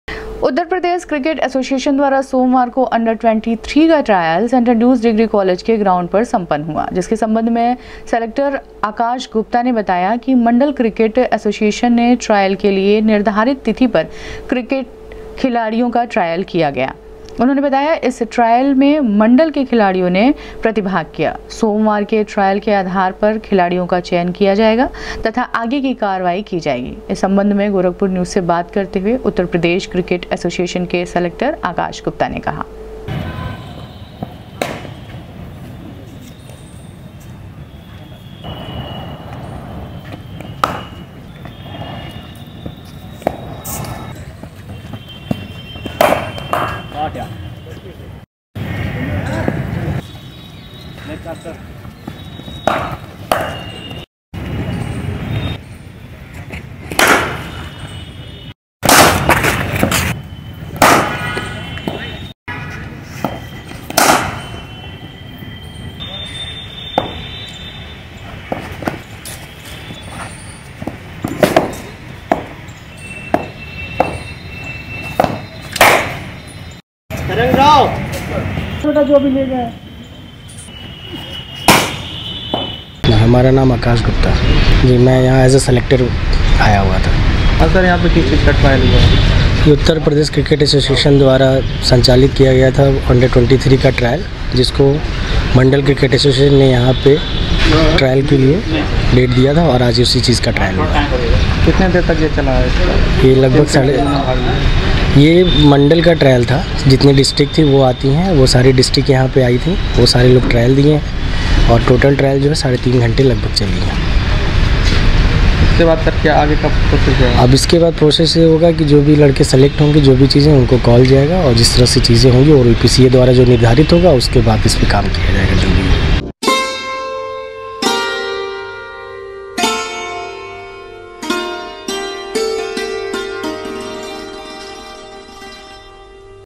उत्तर प्रदेश क्रिकेट एसोसिएशन द्वारा अंडर ट्वेन्टी थ्री का ट्रायल हुआ सम्पन्न,सलेक्टर ने कहा,सोमवार
उत्तर प्रदेश क्रिकेट एसोसिएशन द्वारा अंडर ट्वेन्टी थ्री का ट्रायल हुआ सम्पन्न,सलेक्टर ने कहा,सोमवार को हुए ट्रायल के आधार पर खिलाड़ियों का होगा चयन